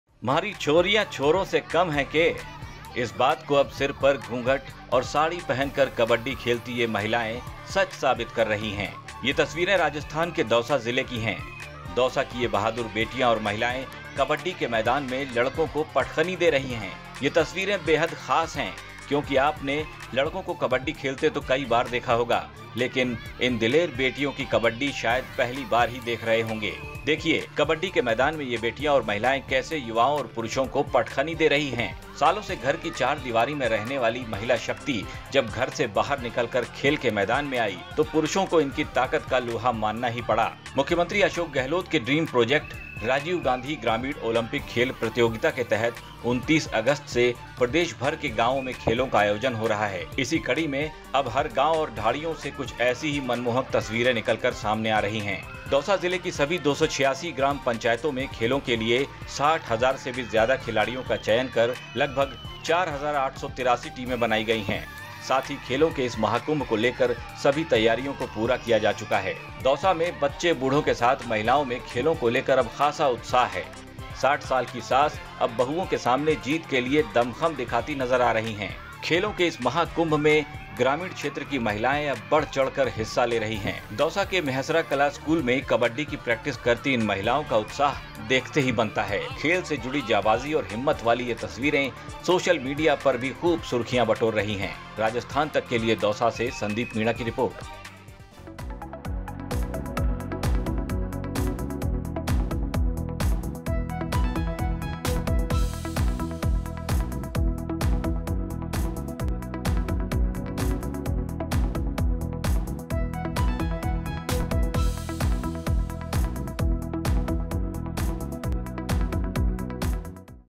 Dausa के गांवों में खेल को लेकर नारी शक्ति में ज़बरदस्त उत्साह ! Rajiv Gandhi Rural Olympics
दौसा ग्रामीण सार्वजनिक मैदानों में खेल नहीं पाने की टीस लिए सालों से घर की चौखट के भीतर घूंघट में रहने वाली ग्रामीण महिलाएं अब ग्रामीण ओलंपिक में दमखम दिखाते हुए पुरुषों के बराबर ताल भी ठोकती नजर आएंगी। यह अधूरा ख्वाब राज्य सरकार की ओर से राजीव गांधी ग्रामीण ओलंपिक खेल प्रतियोगिता के मार्फत पूरा होगा। बहरहाल, गांवों में खेल दिवस पर 29 अगस्त से प्रारंभ होने वाले अनूठे खेलों के प्रति गांव के हर वर्ग के लोगों में भारी उत्साह व उमंग भी है।